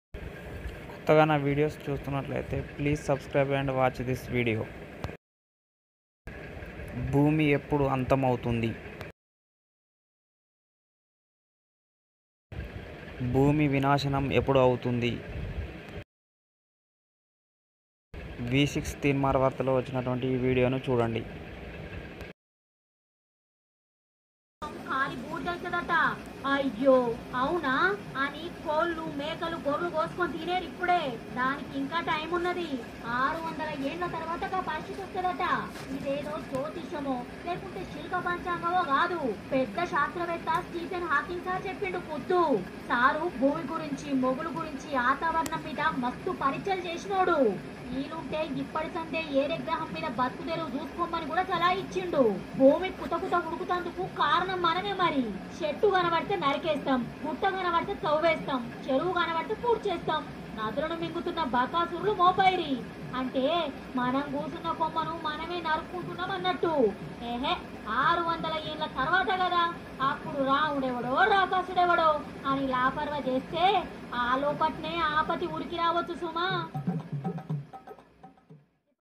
2600 lo Boomi antham avthunda?
Boomi antham epudu, v6 thinmar news lo vachhina article 2600 year lo avthadi anta